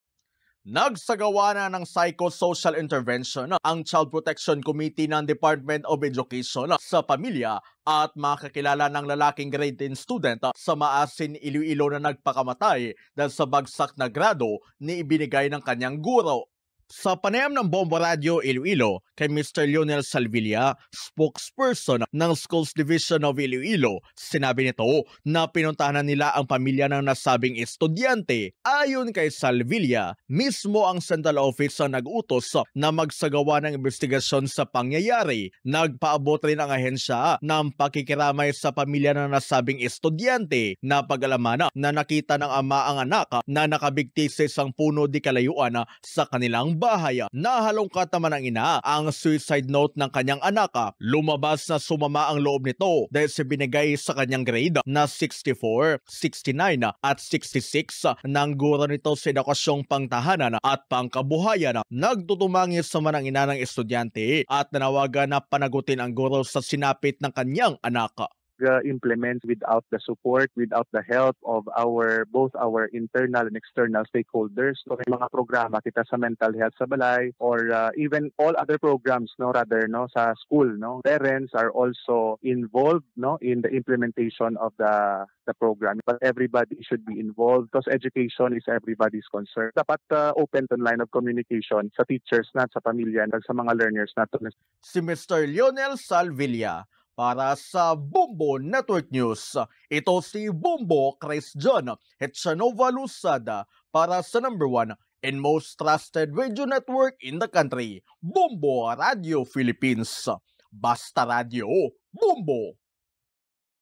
DepEd, nagsagawa na ng imbestigasyon hinggil sa Grade 10 student sa Iloilo na nagpakamatay
DepEd, nagsagawa na ng imbestigasyon hinggil sa Grade 10 student sa Iloilo na nagpakamatay matapos bigyan ng bagsak na grado ng kanyang guro